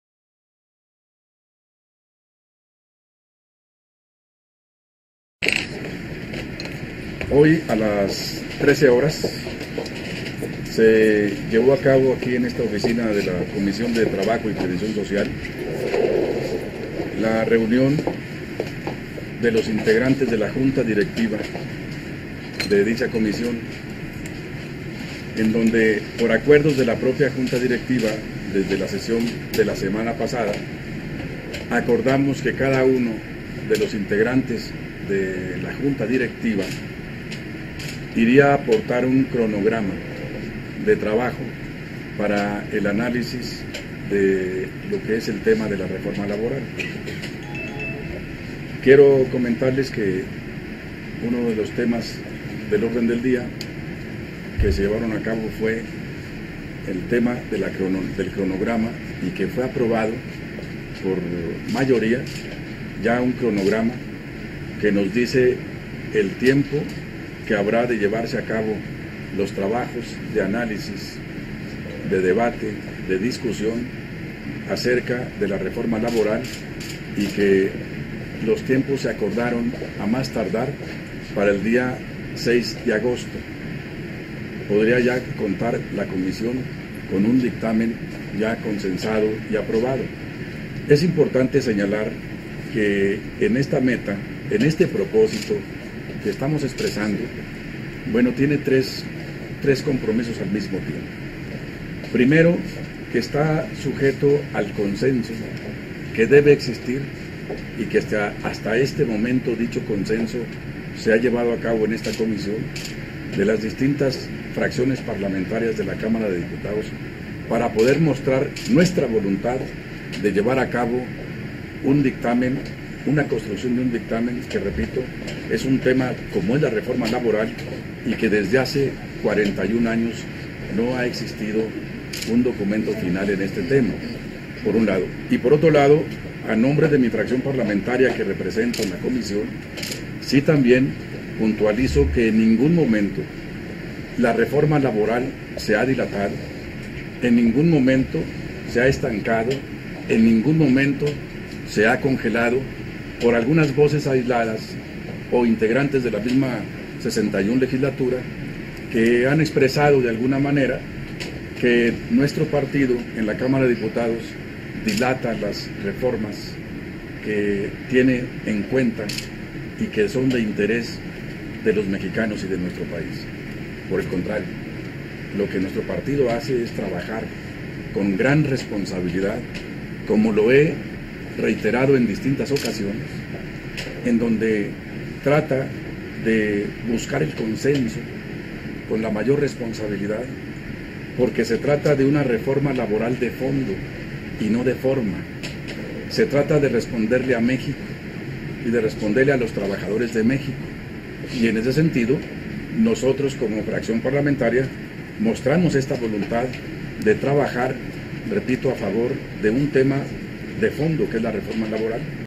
Tendremos el dictamen de la Reforma Laboral el próximo 6 de agosto diputado Tereso Medina
Fue aprobado por la mesa directiva de la Comiisón de Trabajo el calendario de análisis y discusión de la Reforma Laboral,  a más tardar el 6 de agosto próximo esta comisión tendrá un dictamen consensado y aprobado.
Nuestro principal propósito es lograr un concenso de las distintas fracciones parlamentarias, lo que muestra la voluntad de sacar adelante esta reforma, será el documento más importante de los últimos 41 años en materia laboral.
El PRI insiste en sacar adelante estas modificaciones, las acuasaciones que señalan dilación o congelamiento de la reforma laboral son falsas, se trata de una reforma laboral de fondo para responder a las necesidades de los trabajadores y del país. Descartamos cualquier señalamiento de interese partidistas en contra de ese objetivo.